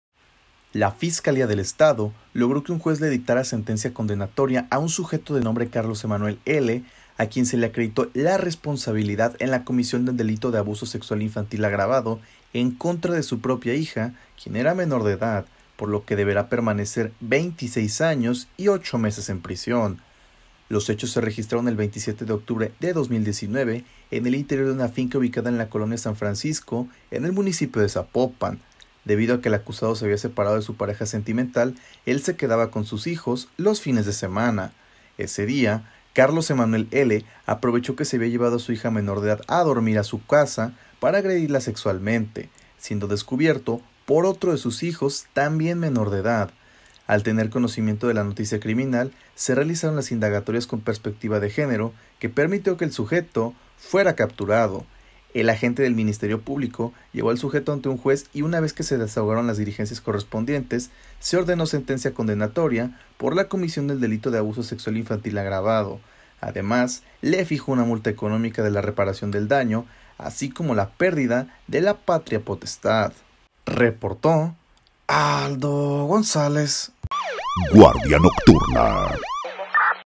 26 años de prisión fue la sentencia para un sujeto que abusó sexualmente de su pequeña hija
26 años de prisión fue la sentencia para un sujeto que abusó sexualmente de su pequeña hija